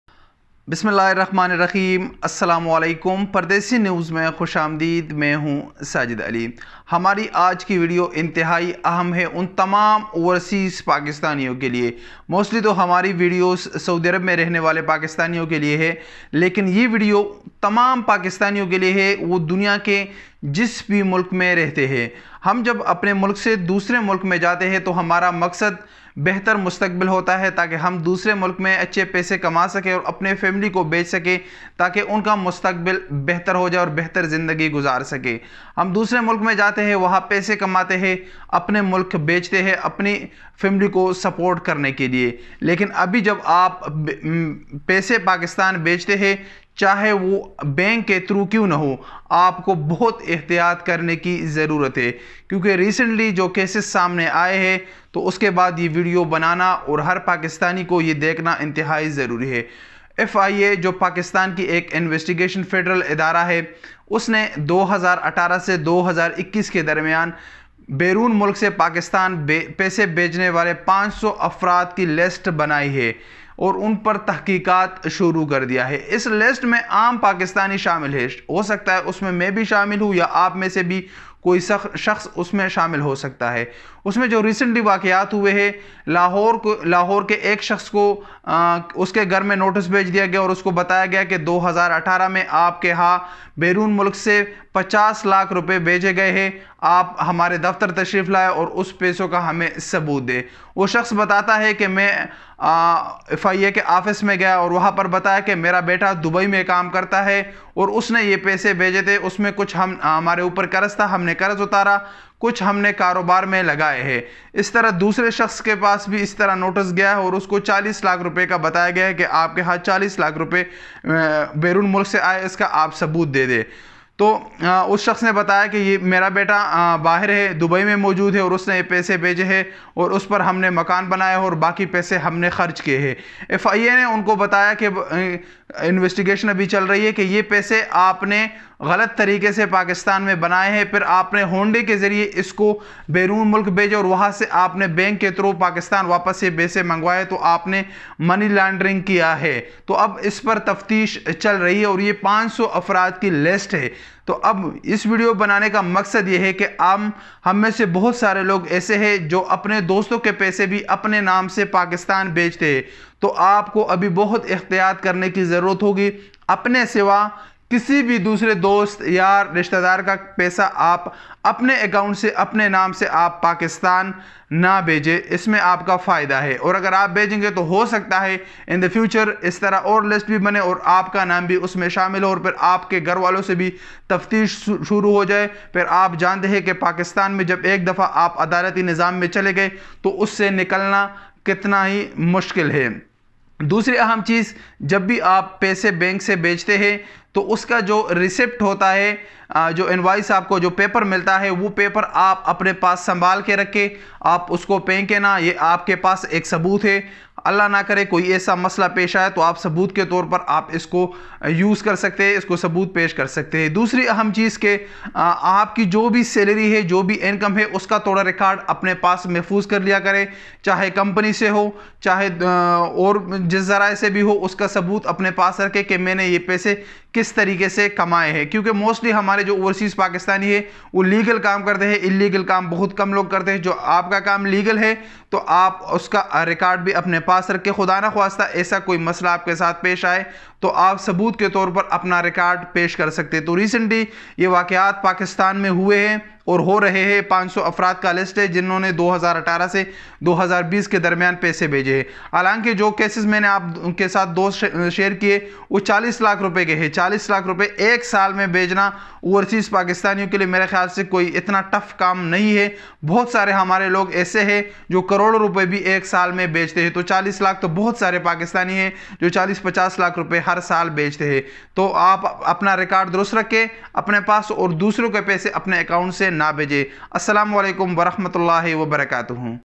Extensive investigation into remittances from overseas Pakistanis | Overseas Pakistani | FIA
This Channel is all about expatriates in Saudi Arabia